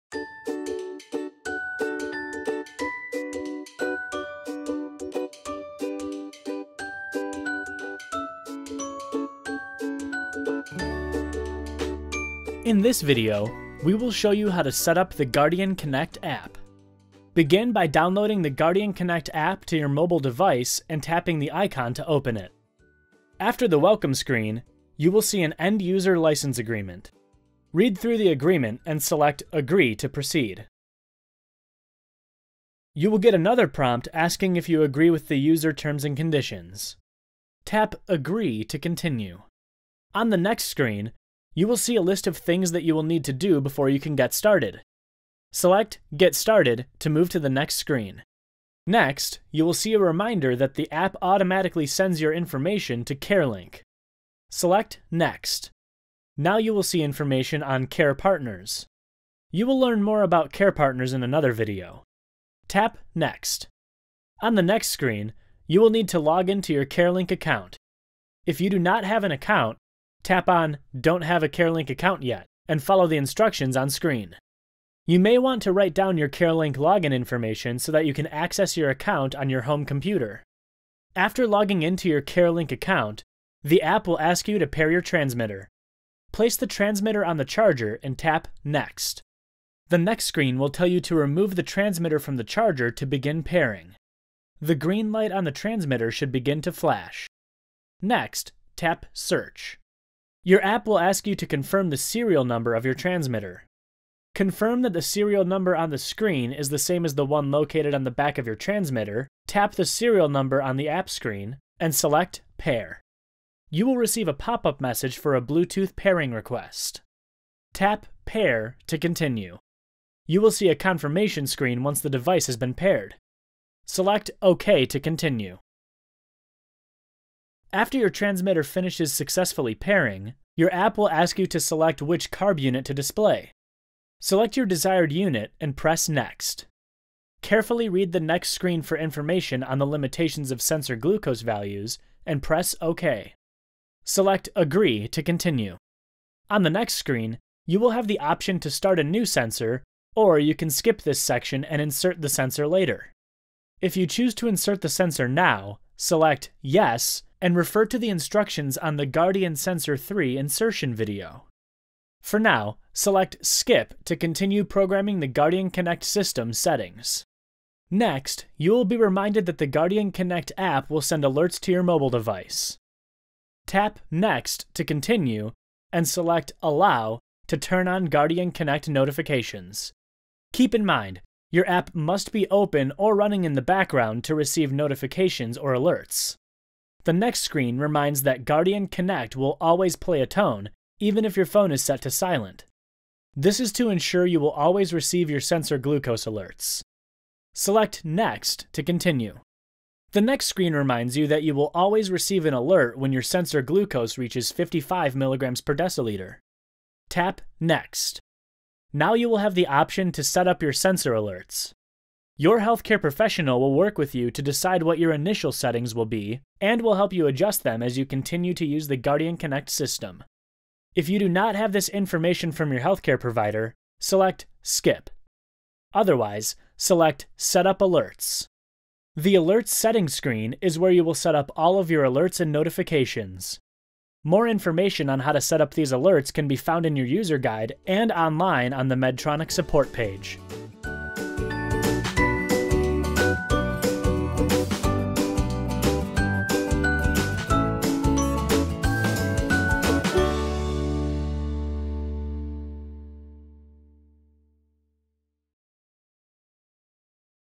How to Set Up the Guardian™ Connect App
In this video, we will walk you through starting up the Guardian™ Connect app system and pairing your transmitter to your app, choosing your preferred carb unit, starting your sensor and more.